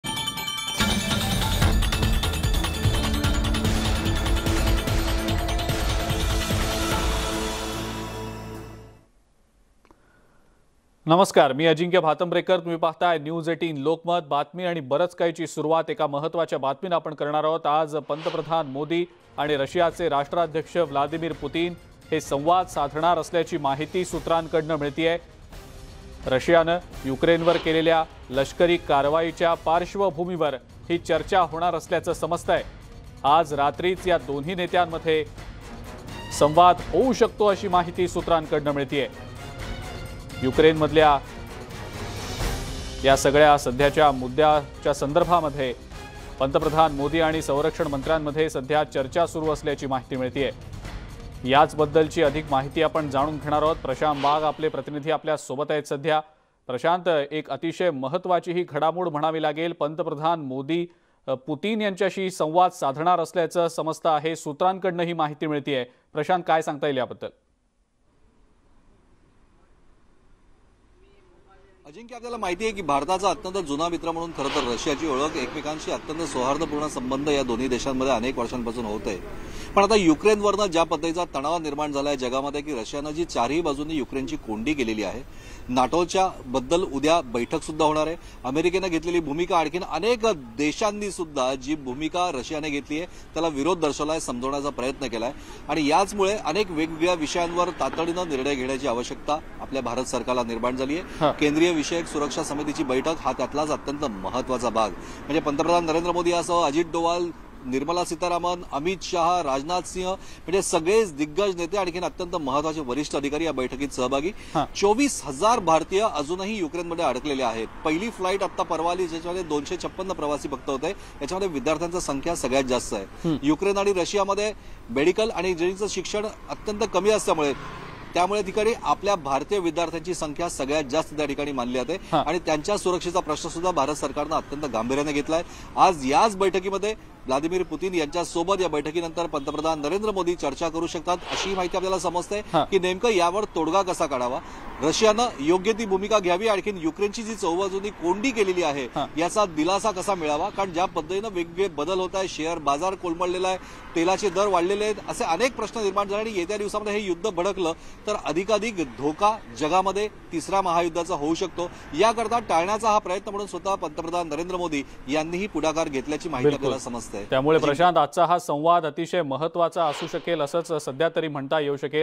Russia Ukraine Crisis | रशिया - युक्रेन युद्धात PM Modi यांची मध्यस्थी? मोदी आणि पुतीन सवांद साधणार
पंतप्रधान मोदी पुतीन यांच्याशी बोलणार असल्याची माहिती सूत्रांकडून मिळत आहे. युक्रेनबाबत आणि युद्धाच्या पार्श्वभूमीवर पुतीन यांच्याशी चर्चा करणार असल्याच सांगण्यात आलेलं आहे. आज रात्री चर्चेची शक्यता, सूत्रांची माहिती